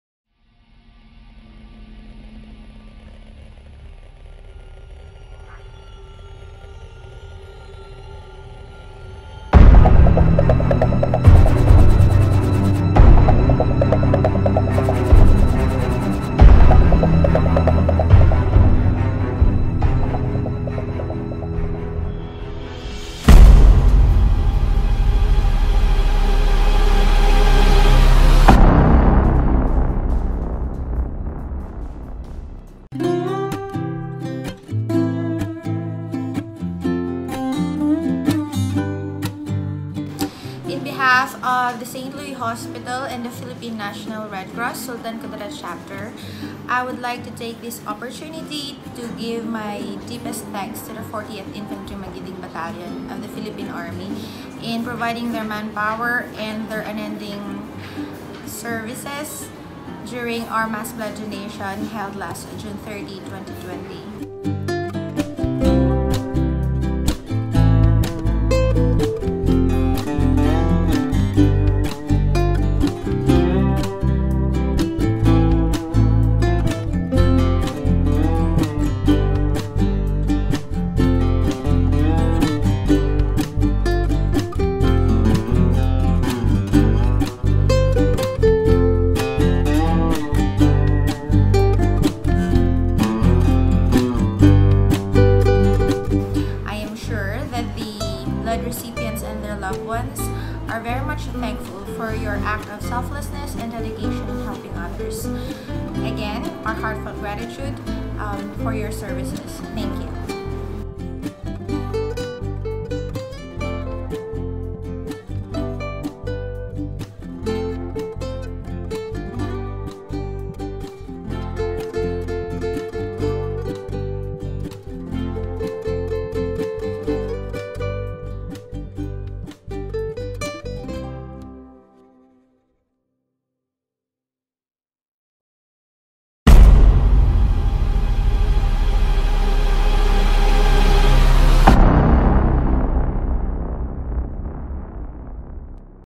𝗠𝗔𝗚𝗜𝗧𝗜𝗡𝗚 𝗧𝗥𝗢𝗢𝗣𝗘𝗥𝗦 𝗗𝗢𝗡𝗔𝗧𝗘𝗦 𝗕𝗟𝗢𝗢𝗗 @ ST. LOUIS TACURONG CITY
𝗠𝗔𝗚𝗜𝗧𝗜𝗡𝗚 𝗧𝗥𝗢𝗢𝗣𝗘𝗥𝗦 𝗗𝗢𝗡𝗔𝗧𝗘𝗦 𝗕𝗟𝗢𝗢𝗗

40th Infantry (MAGITING) Battalion headed by LTC ROGELIO S GABI INF (GSC) PA participated in a bloodletting activity at St Louis Hospital, Tacurong City, Sultan Kudarat on June 30, 2020.

Bloodletting activity was made possible by the staff of St Louis Hospital, Tacurong City thru Dr. Vanessa Del Muro and in coordination with the Philippine National Red Cross, Sultan Kudarat Chapter.

The blood donors for this activity are the CAA and SCAA under the supervision of the Delta Company of 40IB. A total of twenty (20) units of blood was donated and are expected to lengthen the lives of the grateful recipient.

This activity aims to increase the depleting number of available bloods units in the blood bank which is fast decreasing due to the effect of covid-19. “Our hospital is in need of blood units almost everyday, the demand for blood units in every hospital does not decrease but the number of blood donors significantly decreased due to the effect of covid-19.” Said Dr. Del Muro.

“The efforts of St Louis Hospital in Tacurong City will always remind us of our civic duty to help each other in times of crisis. “LTC GABI said.

“The precious blood donated by of CAA and SCAA will be used by an unknown recipient but our men will always be grateful knowing that they will soon save a life. LTC GABI added.
Such concerted efforts exhibits one of the innate characters of the Filipinos, the Bayanihan. The Magiting Troopers will always be in the service of the people, not just being part of the job but rather our passion as we go along our journey for a peaceful and development Mindanao.

Apollo - Alpha Company, 40IB, 6ID, PA
Bravo Barracuda
Cerberus Troops
St. Louis Hospital - Tacurong City

Let’s make peace viral mga Ka-Magiting! 🙂